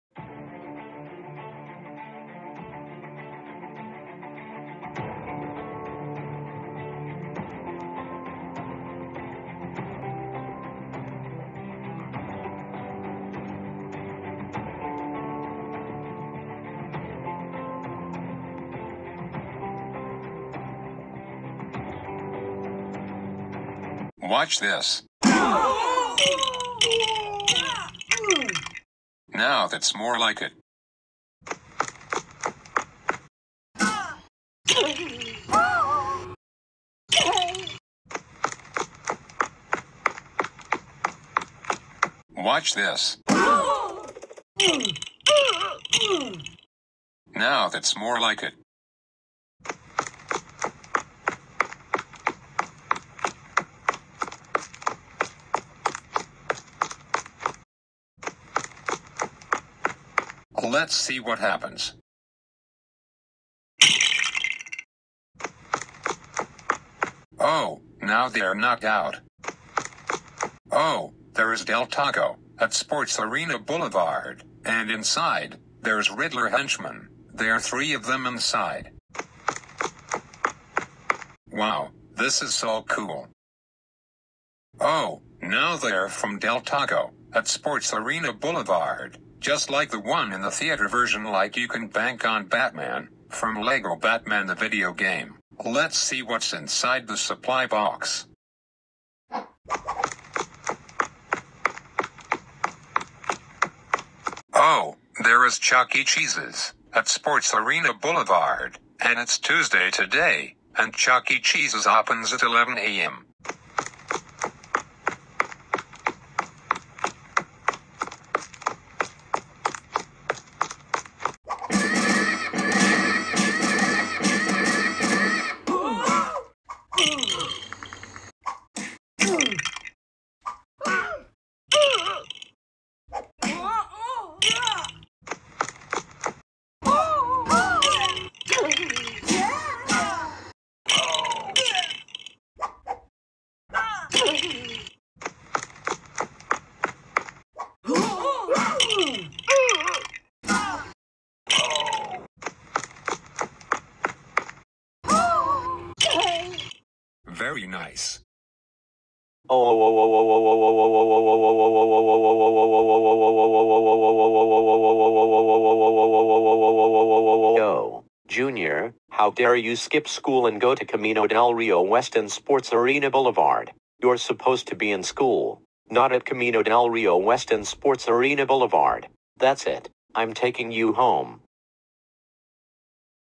Junior goes to Camino Del Rio W and Sports Arena Blvd and gets in trouble
Junior skips school and goes to Camino Del Rio W and Sports Arena Blvd and Gets Grounded